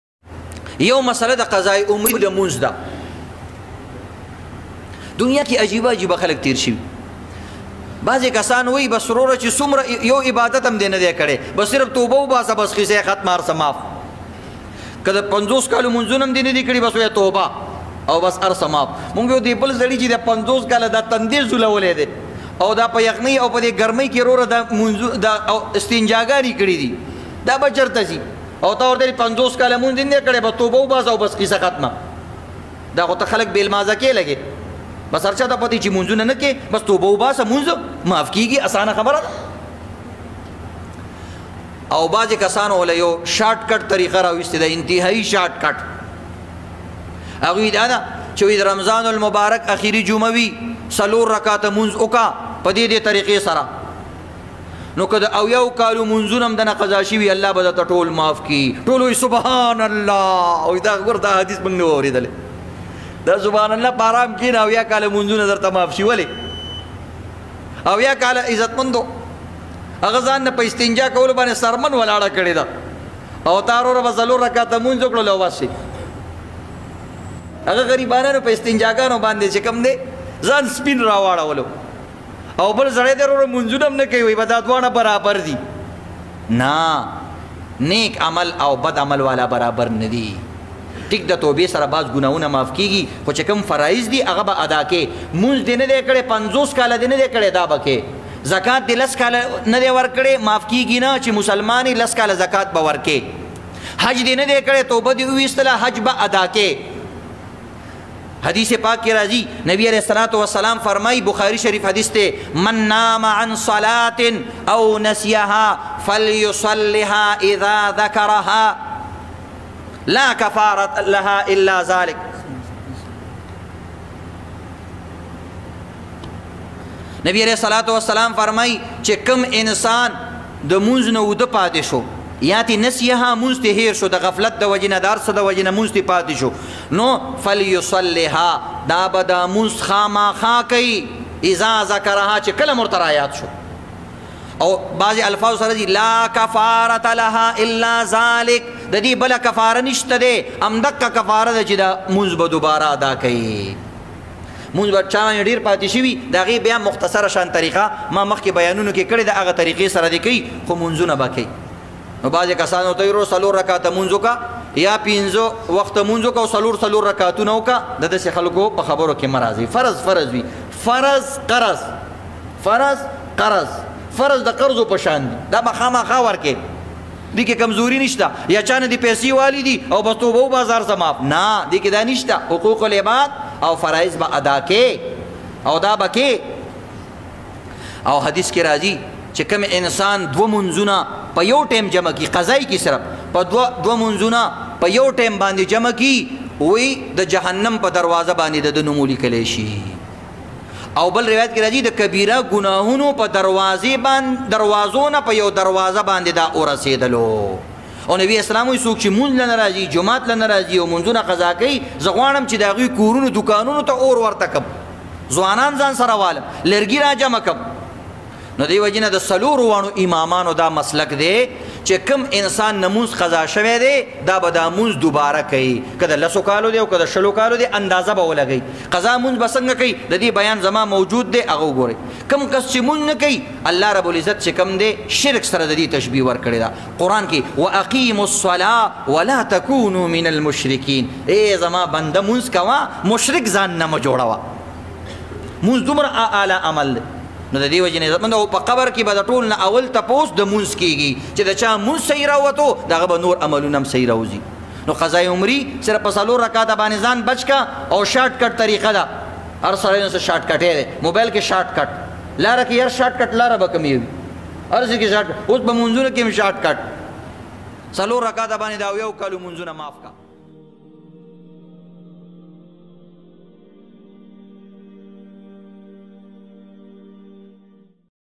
Qaza e umri namaz -Monz (pashto) | قضائے عمری نماز | Mufti Raheem Dad | Saadi Media
qaza e umri namaz Jaiz hi ?

کیا رمضان کے آخری جمعے  میں 4 رکعت نماز 70 سال کے نماز کے برابر ہوتی ہے ؟

کیا رمضان میں چار رکعت قضائے عمری بدعت ہے ؟

Pa Islam k Qaza e Umri Monz shta ?

pa Ramaza pa Akeri juma bandy 4 rakat da 70 kalo monz barbr v ?

مفتی رحیم داد دامت برکاتہم

شیخ الحدیث مولانا حسن جان شھید کیا کہتے ہے قضائے عمری کے بارے میں👇